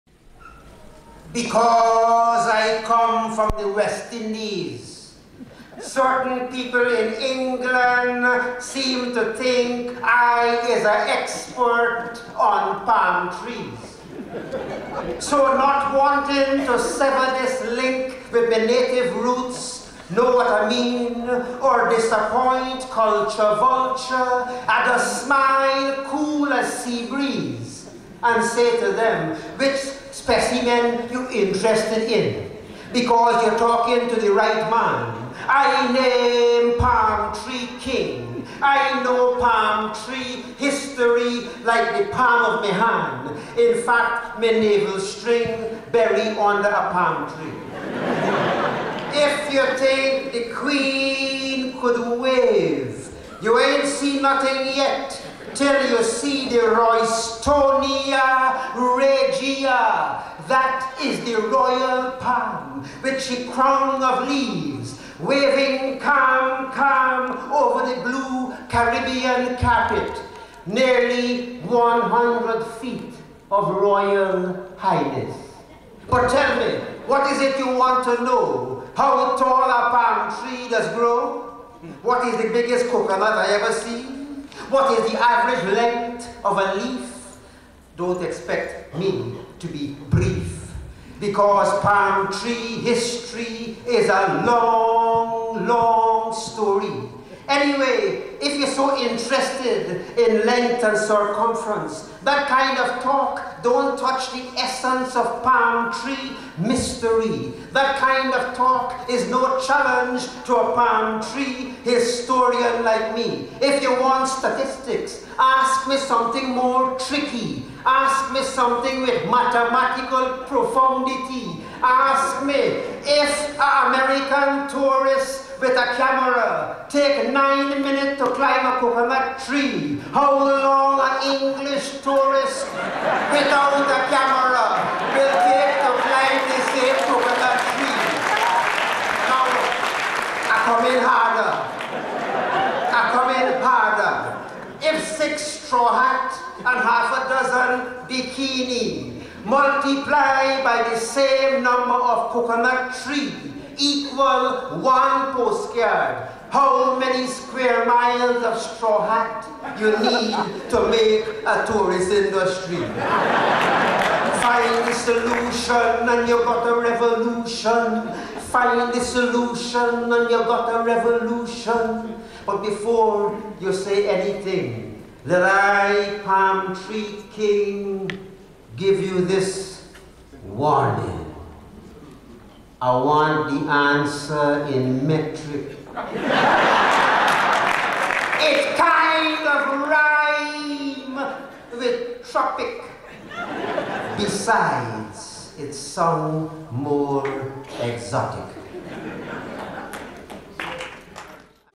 A Great Poet
A Dynamic voice and a Dynamic performer, Our own
John Agard - Palm Tree King.